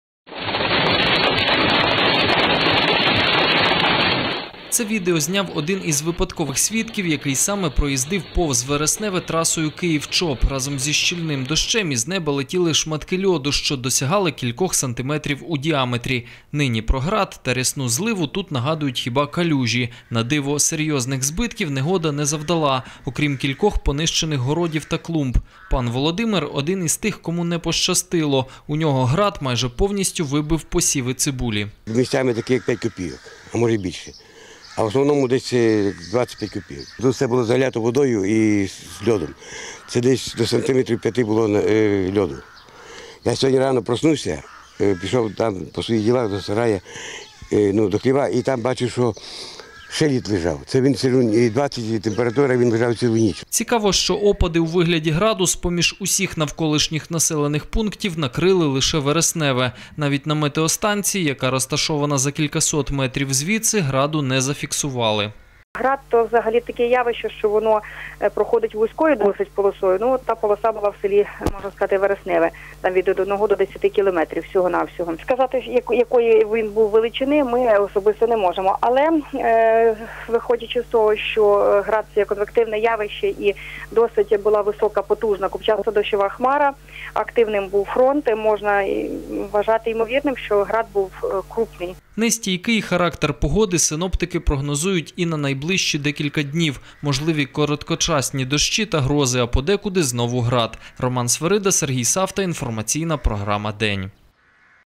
Град не дістав до Рівного, але поцілив у Вересневе
Град розміром із перепелине яйце випав вчора у селі Вересневе, що неподалік Рівного. Серйозної шкоди опади не наробили. За винятком кількох винищених городів. Втім, незвично крупний град став темою для обговорень у соцмережах. Тему продовжить - Роман Свирида.